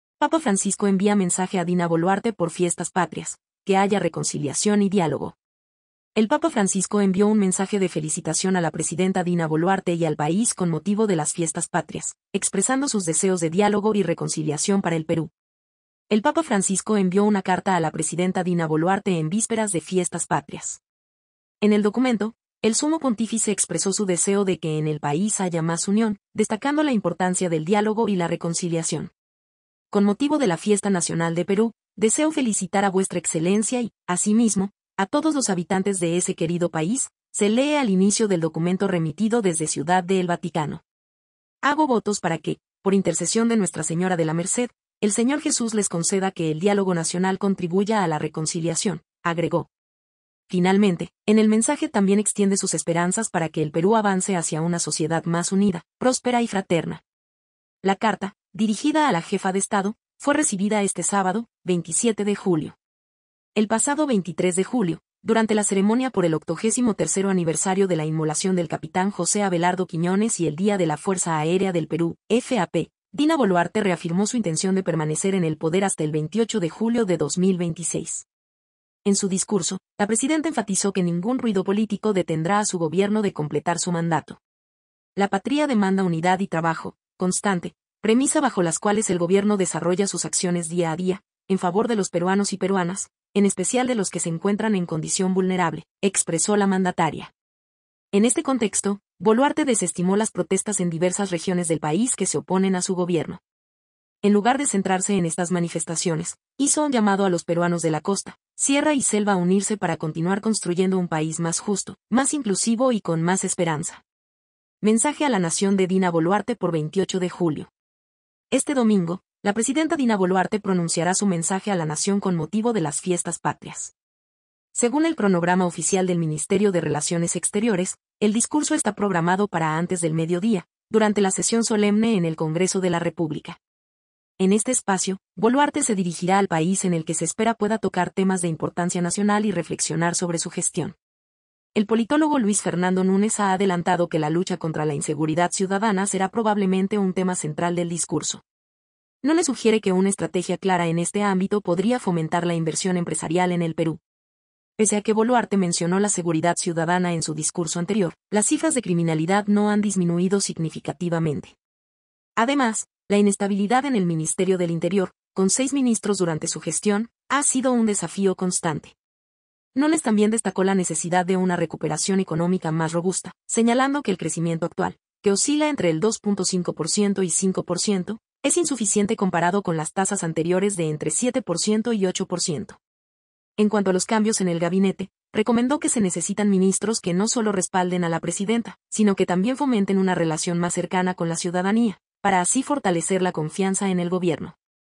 Papa Francisco envía saludos por Fiestas Patrias y pide un “diálogo nacional” a Dina Boluarte
La presidenta Dina Boluarte recibió la misiva del Sumo Pontífice en la que la saluda junto al resto de peruanos por este 28 de julio.